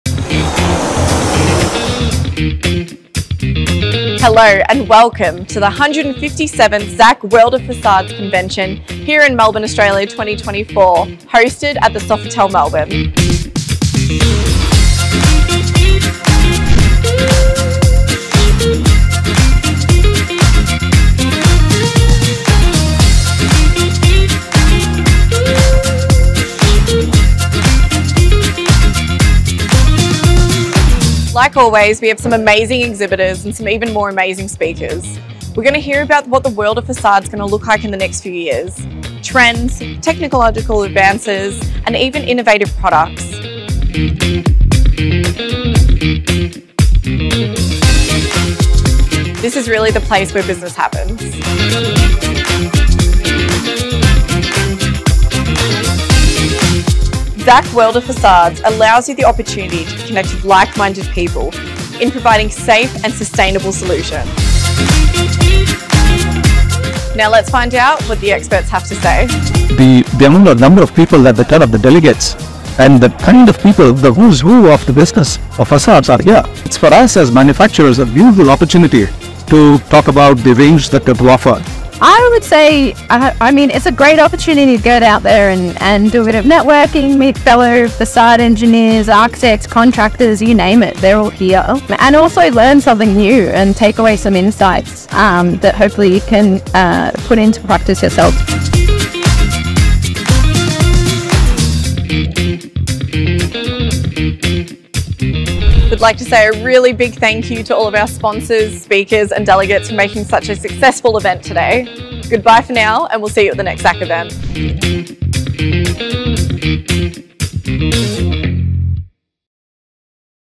Zak World of Façades Australia - Melbourne 2024 Snippet
Snippet from the 157th global edition of the Zak World of Façades event held in Melbourne on 19th September 2024 at Sofitel Melbourne On Collins, Melbourne.